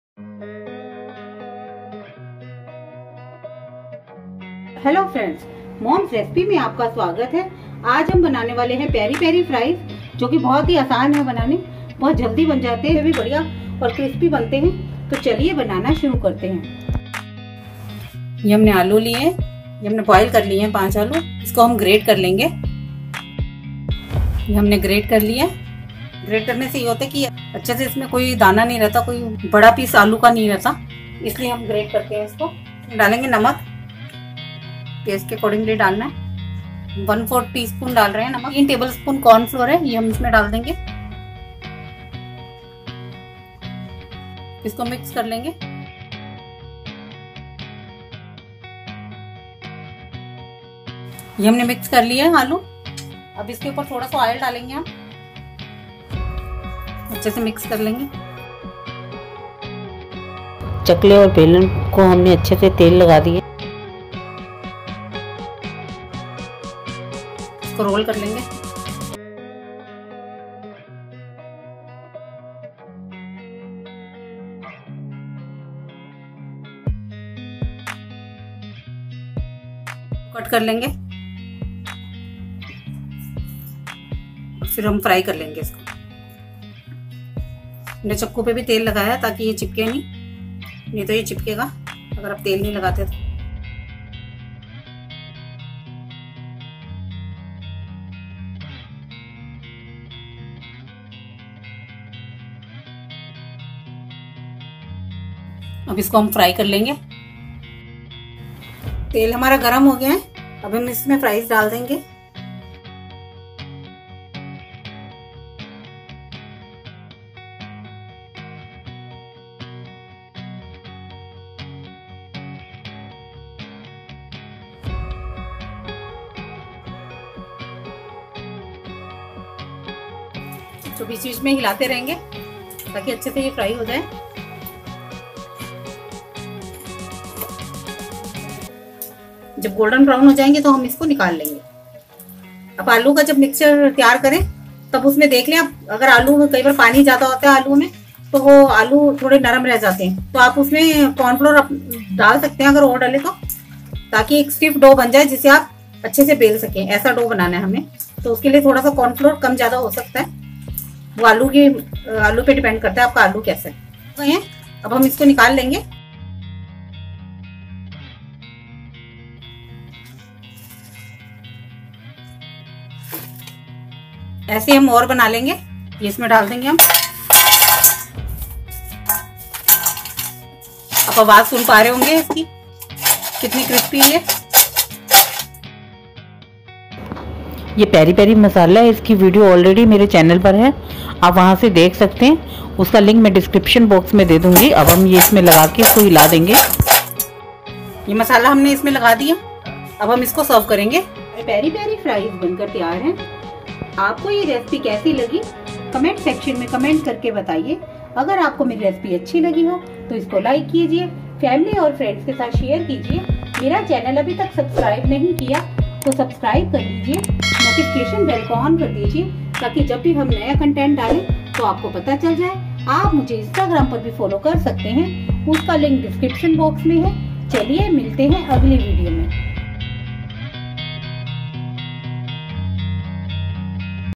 Peri Peri Fries | Homemade French Fries | Crispy restaurant style peri peri fries
Hi friends , Aaj hum restaurant style peri peri fries banane wale hai .French  fries ko hum peri peri masala se coat krenge jisse yeh bahut hi badiya banegi.

Peri Peri Fries recipe is unique method of making fries . We are making fries with different method so that it can be more crispy and stay crispy for long time. Peri Peri fries will be luscious with seasoning of peri peri masala.You must try this recipe once you will like it.

Ingredients :
Boiled Potato - 5
Salt -1/4tsp
Corn Flour -3tbsp
Peri Peri Masala  as required

notified.